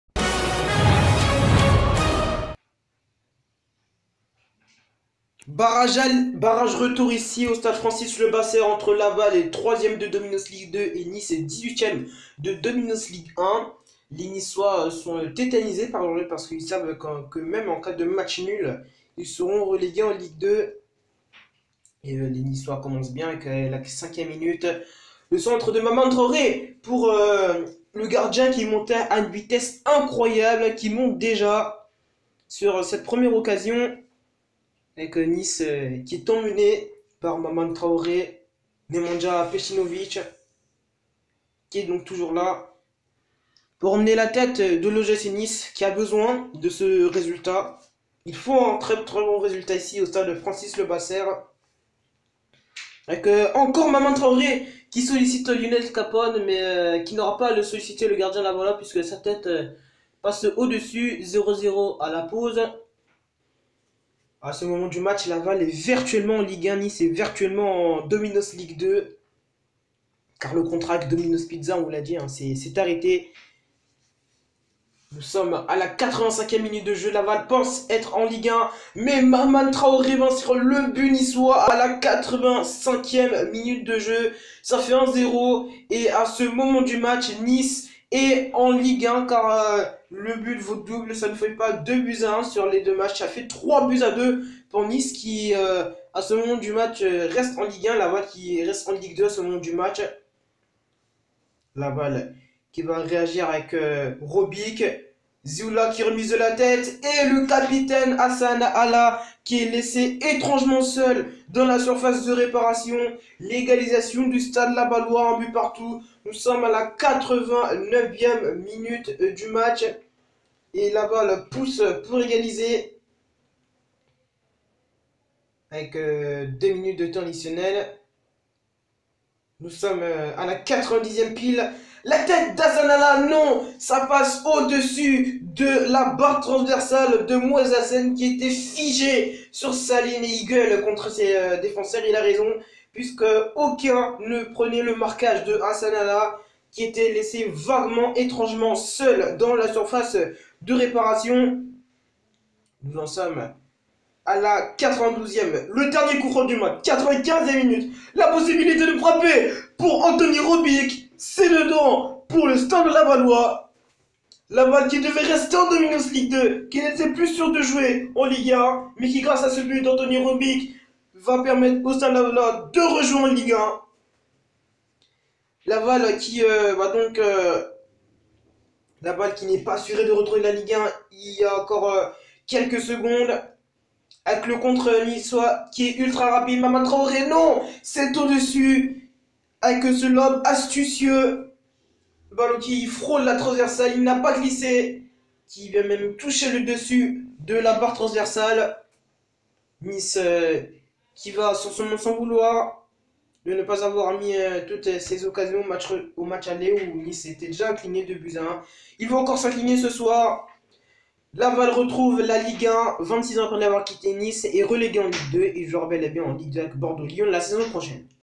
Stade Lavallois MFC - OGC Nice (2-1) - Résumé Barrage Retour Domino's Ligue 1 - (LAVAL - OGCN)
Revivez les meilleurs moments de Stade Lavallois MFC - OGC Nice (2-1) en vidéo

Barrage Domino's Ligue 1 - Saison 2014/2015 - Barrage Retour

Stade Francis Le-Basser - Samedi 21 Octobre 2017

Buteurs : Mahamane TRAORÉ (85' - OGC Nice) / Hassane ALLA (90' - Stade Lavallois MFC) / Anthony ROBIC (90+5' - Stade Lavallois MFC)